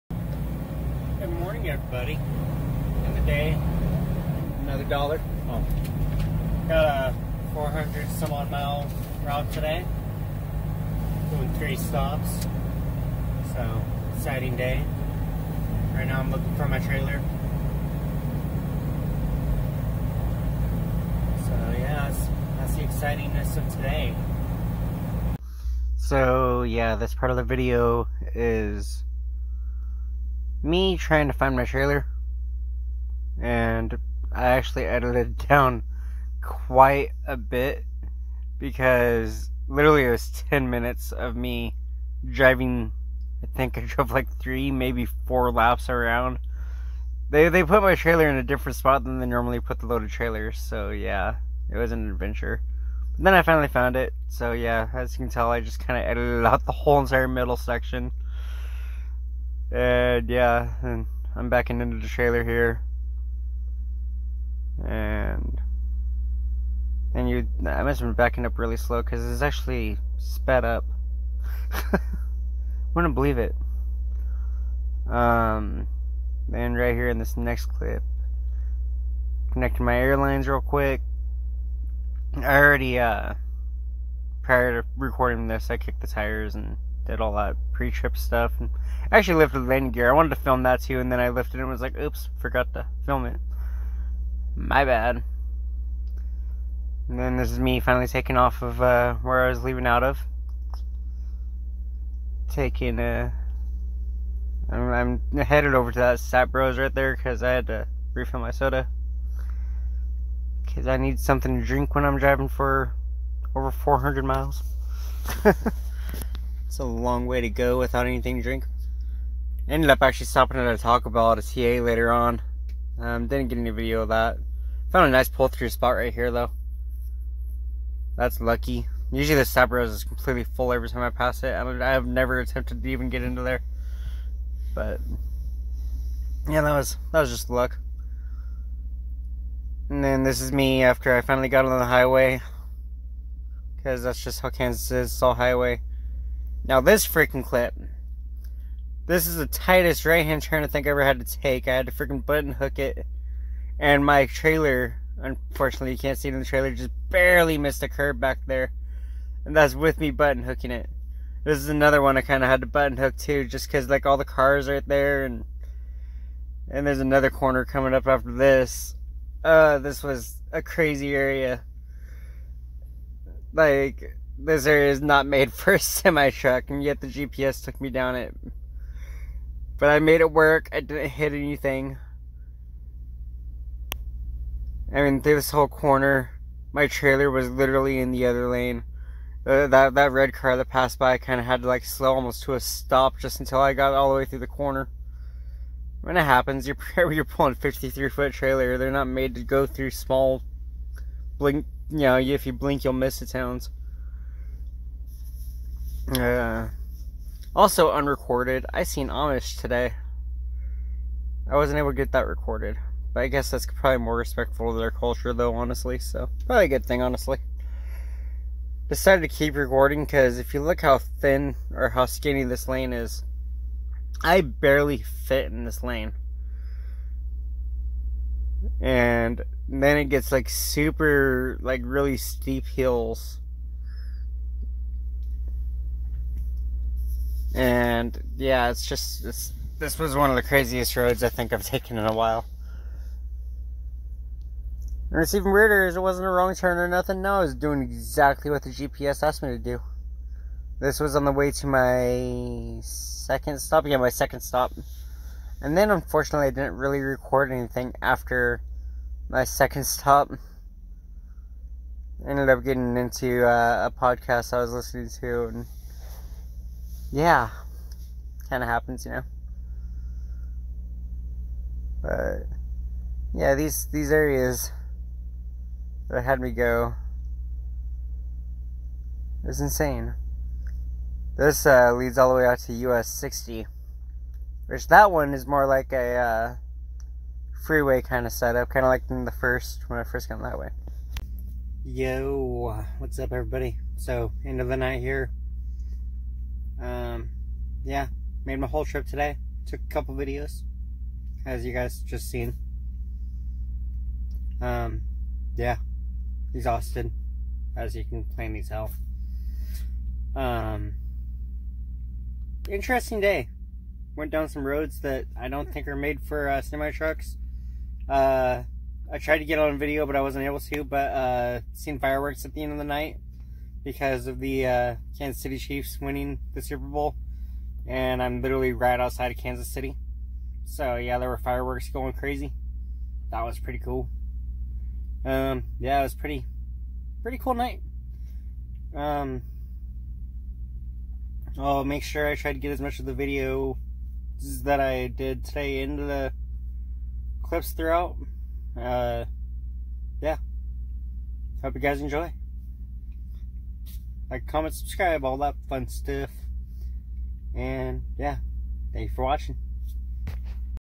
Killjoy's Trucking Vlog #3: This Road is Not Made For Trucks
Join us as our rookie truck driver goes down roads not made for trucks